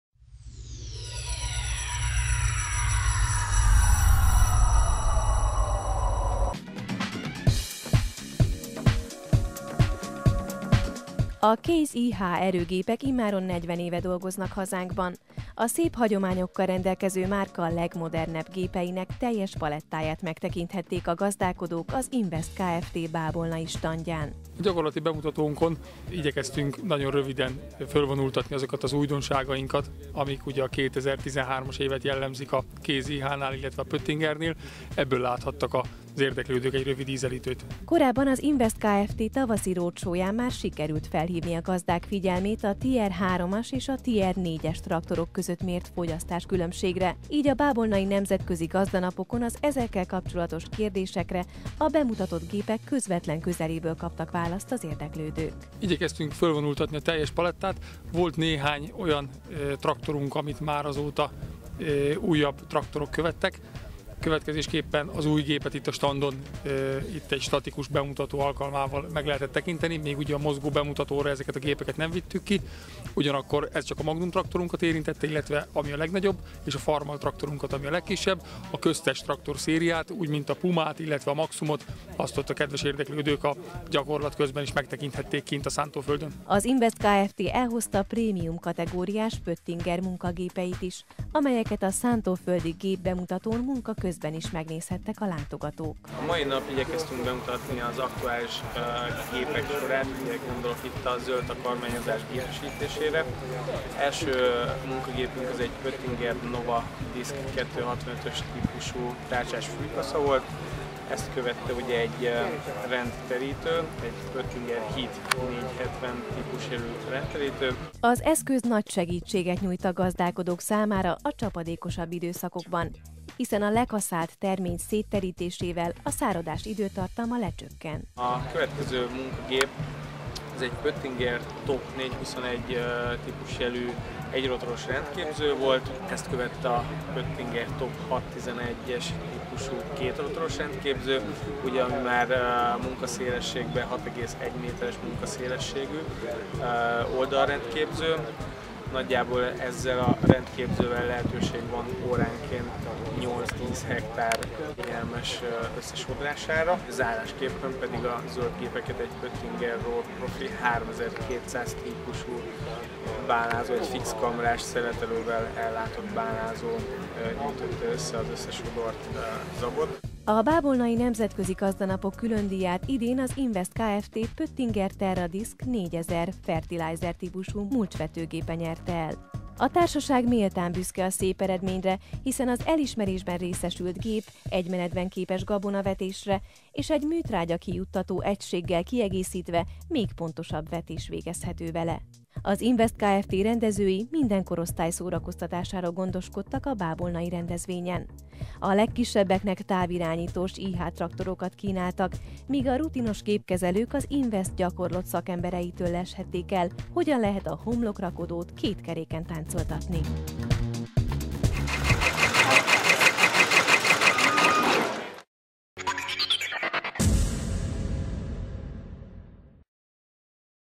Az Invest Kft. óriási standdal jelent meg a Bábolnai Gazdanapokon - CASE IH, Pöttinger
Az Invest Kft. a nemzetközi gazdanapokon látványos szántóföldi bemutatók keretében mutatta be prémium kategóriás erőgépeit munkavégzés közben. Pöttinger Nova tárcsás fűkaszát, rendterítőt, a Pöttinger TOP széria rendképzőket és bálázót is láthattak az érdeklődők.